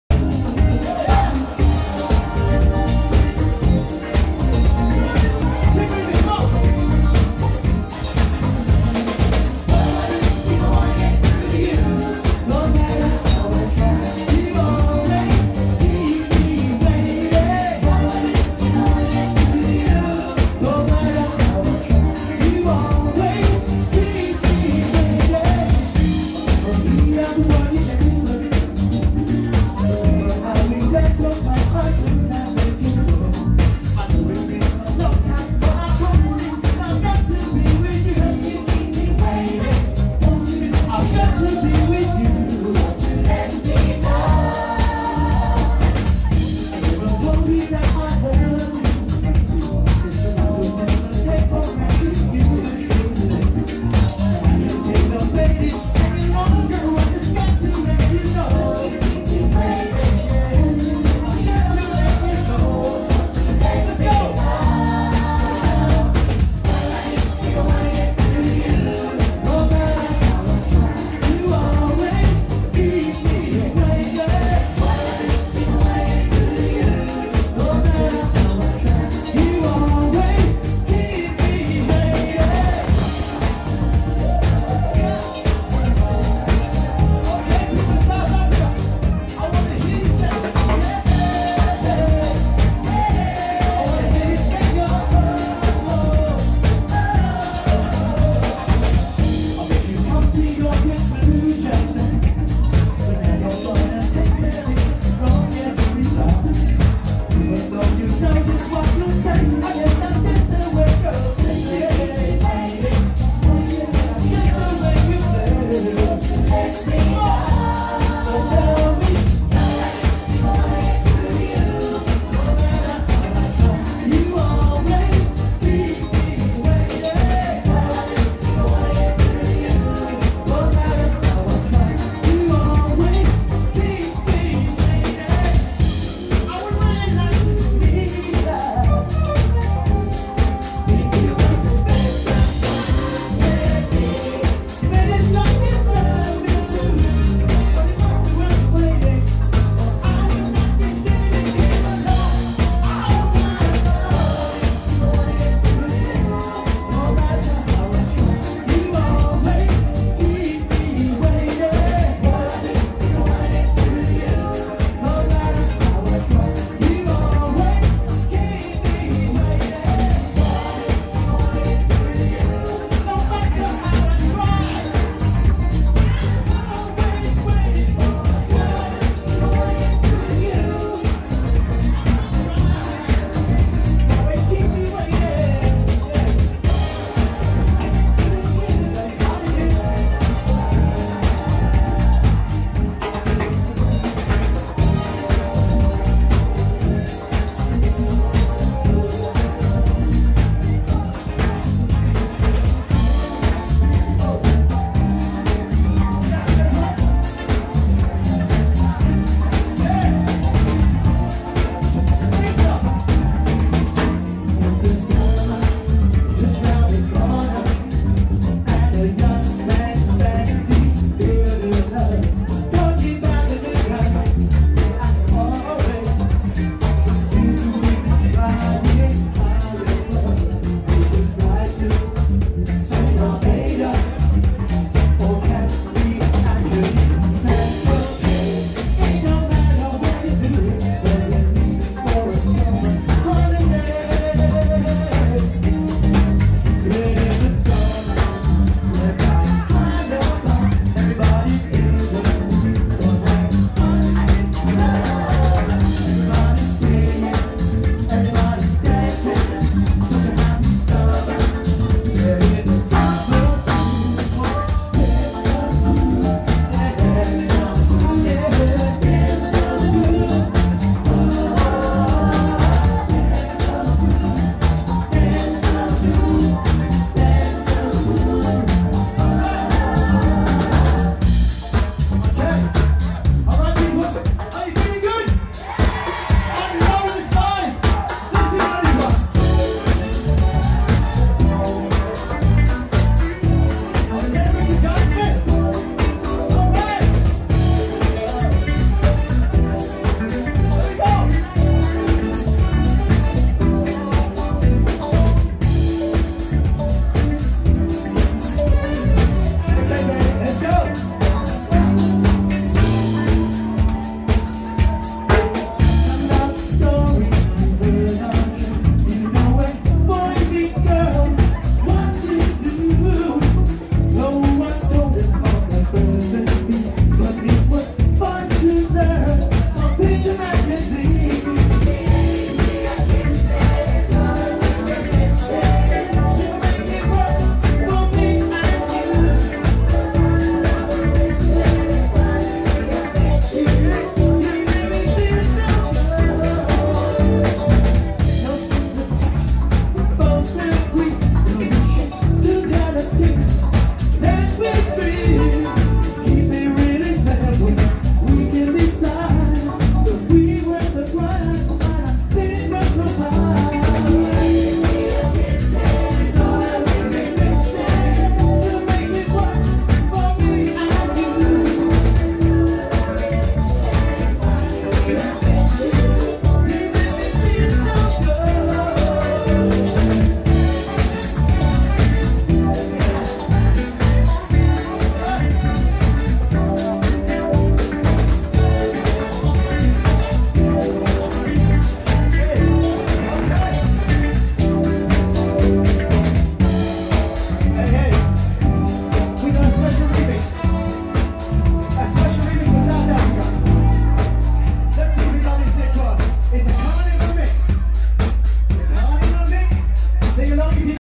Phil Fearon live Love Jazz Cruise_2020
Phil Fearon backed by Jabaz

Band:

TJ-Percs/Vocal(md)
Dillan-Keys
Eddy Strings-Guitars
Alistair-Bass